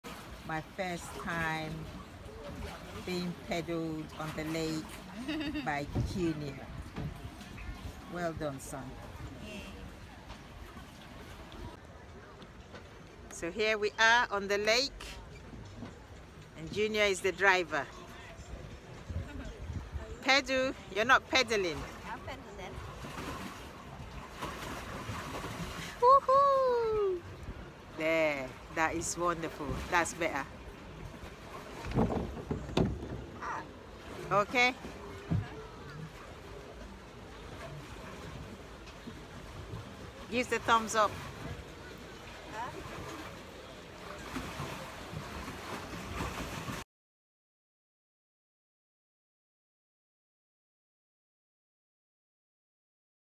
Crystal Palace Boat Lake
We went on a Christian archaeology group tour at Crystal Palace Park where we learnt about the dinosaurs and how they got extinct etc. We ended the day with a boat ride.